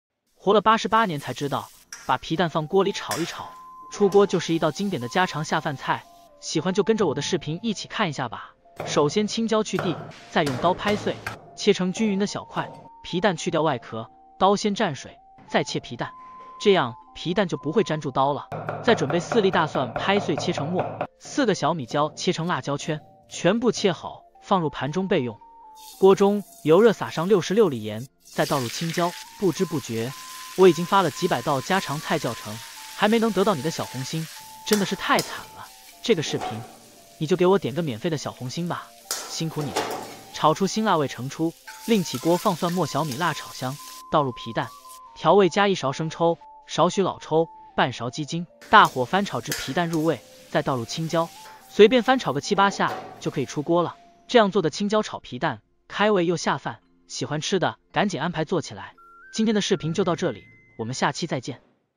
活了88年才知道，把皮蛋放锅里炒一炒，出锅就是一道经典的下饭菜
天南海北家乡味